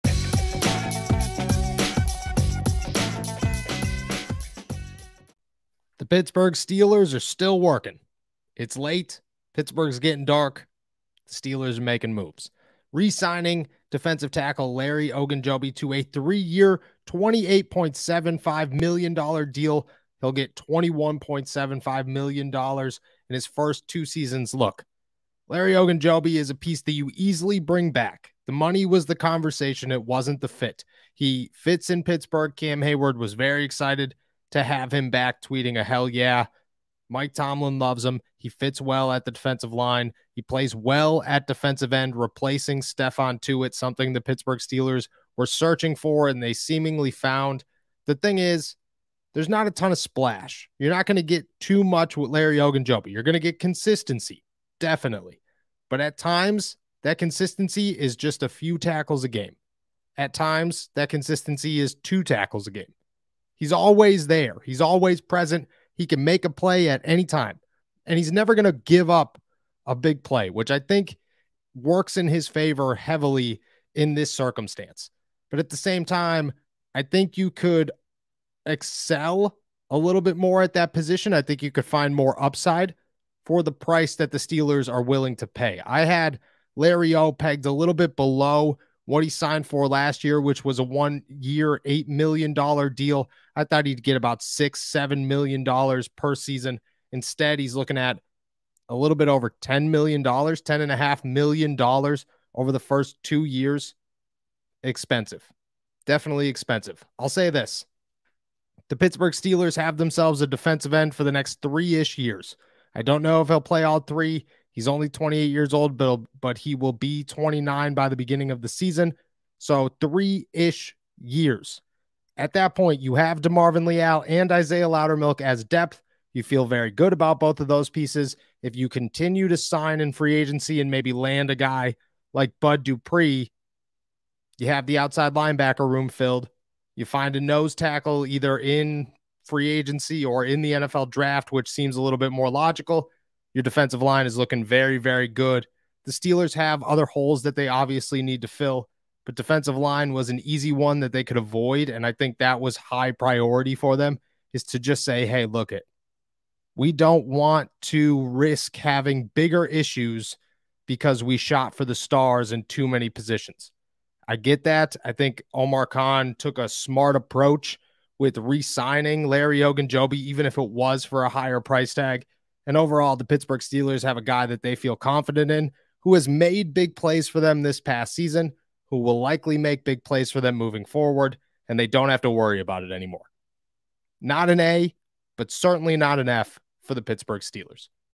Steelers Re-Sign DE Larry Ogunjobi
The Pittsburgh Steelers are bringing back defensive lineman Larry Ogunjobi.

Check Out All These Awesome Promos!